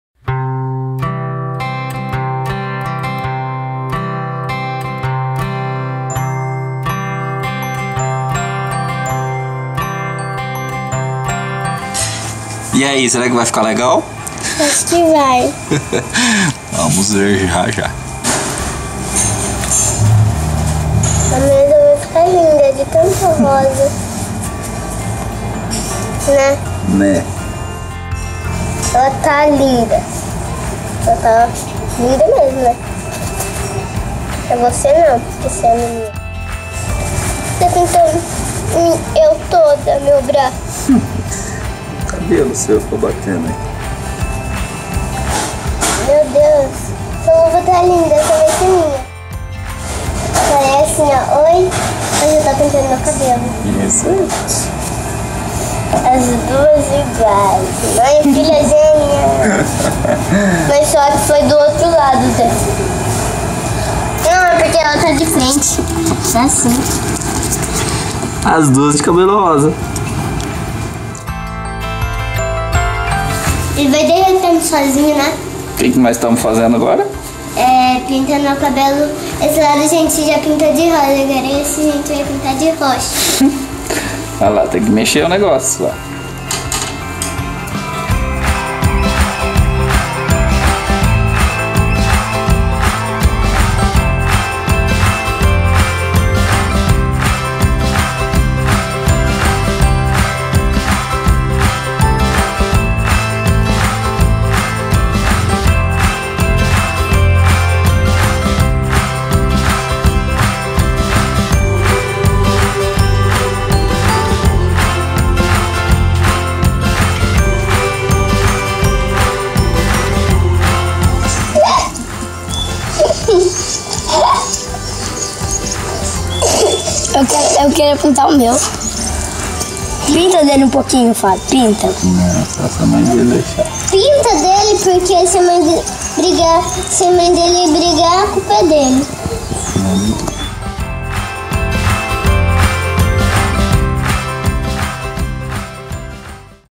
Papai pintando cabelo da filha com papel crepon
Inventei de pintar umas mechas de cabelo da minha loirinha, deixei ela escolher as cores e mãos a obra.

Nunca tinha feito isso em minha vida, então me deem um desconto, hahaha.

O que interessa é que ela gostou, esta foi a intenção, então ta ótimo :D

04/03/2017

by Fábio Toneli